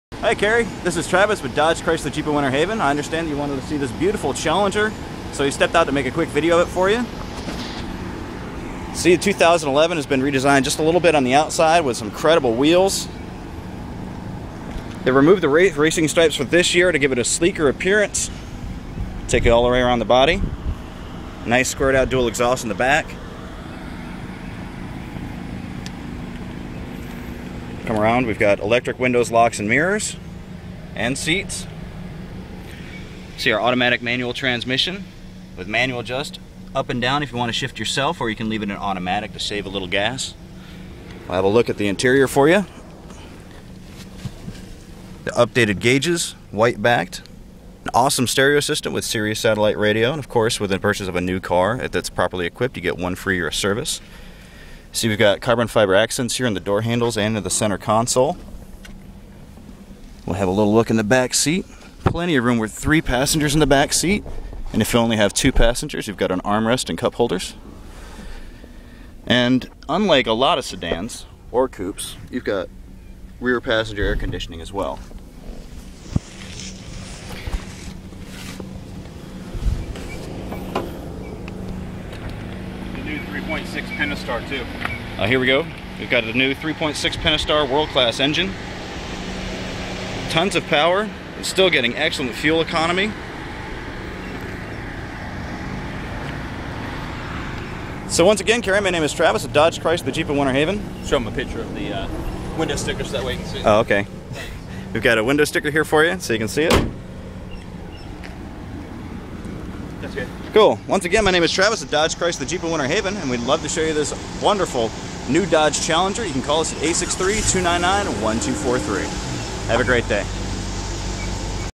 Cary's awesome 2011 Dodge Challenger!
Dodge Chrysler Jeep of Winter Haven is proud to show you this new Challenger!